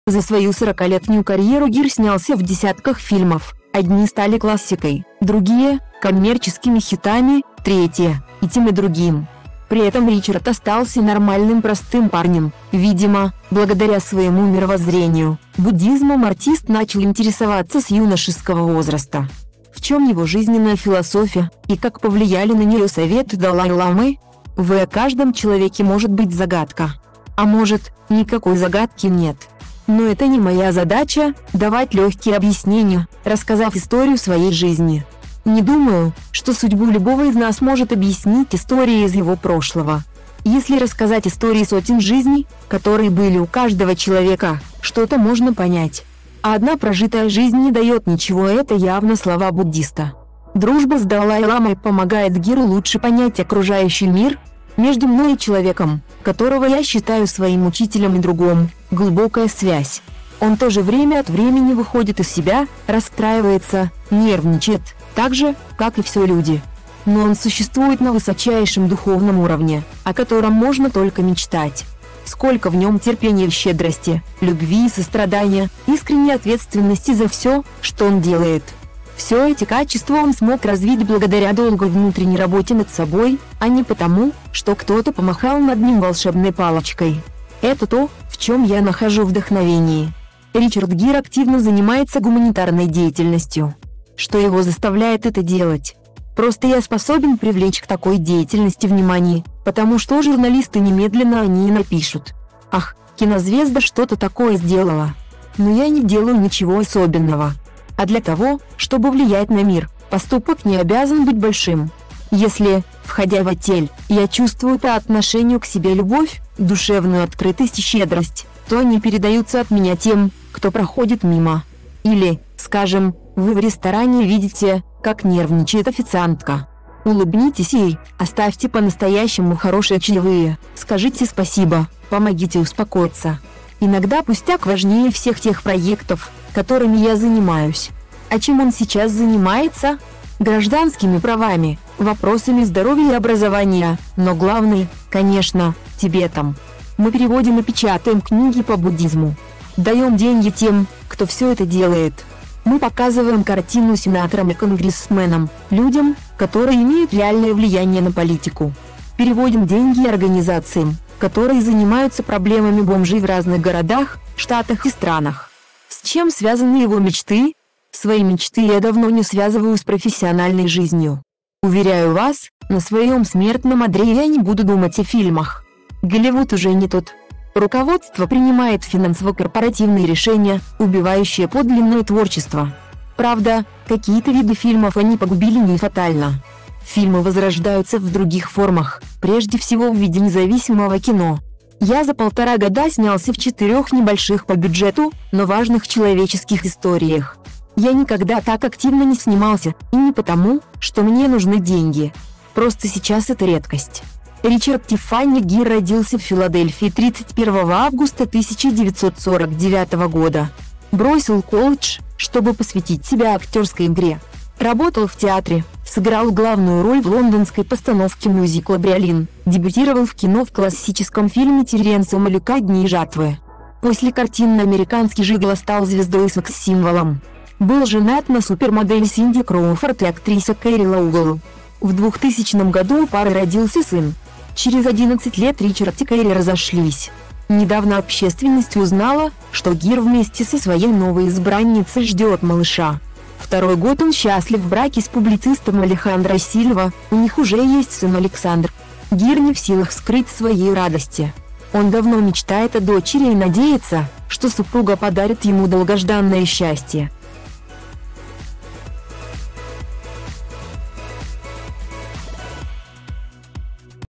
Ричард Гир: Я чувствую боль...
За свою сорокалетнюю карьеру Гир снялся в десятках фильмов, одни стали классикой, другие — коммерческими хитами, третьи — и тем и другим. При этом Ричард остался нормальным простым парнем — видимо, благодаря своему мировоззрению: буддизмом артист начал интересоваться с юношеского возраста. В чём его жизненная философия, и как повлияли на неё советы далай-ламы?  "В каждом человеке может быть загадка. А может, никакой загадки и нет. Но это не моя задача — давать лёгкие объяснения, рассказав историю своей жизни. Не думаю, что судьбу любого из нас может объяснить история из его прошлого. Если рассказать истории сотен жизней, которые были у каждого человека, что-то можно понять. А одна прожитая жизнь не даёт ничего!"  Это явно слова буддиста.